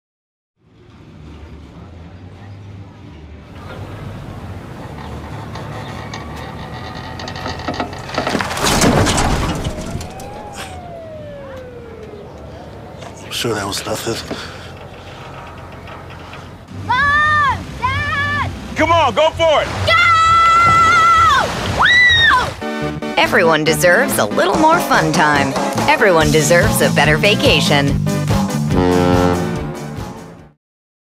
New Carnival Roller Coaster Commercial - CruiseGuy.com
Another Carnival Cruise Lines' video in their Land vs Sea ad campaign. Not as good as first one when bear attacked the car.